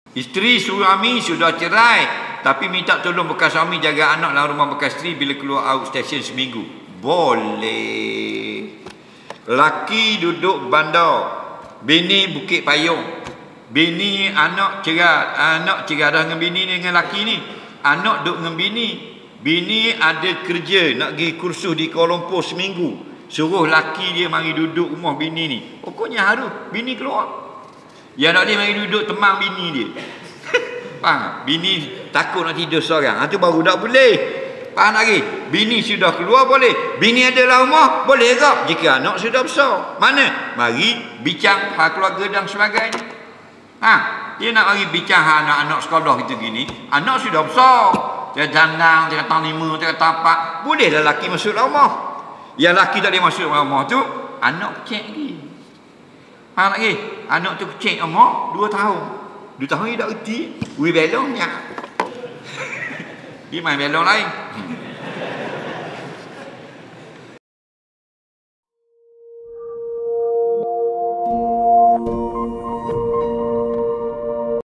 Minta Bekas Suami Jaga Anak - Ustaz Azhar Idrus
📆 14 September 2023 | Khamis
⏰ 07.45 malam
🕌 Masjid Padang Mengkuang, Bukit Payung, Terengganu
📖 Kitab : Safinatu An Naja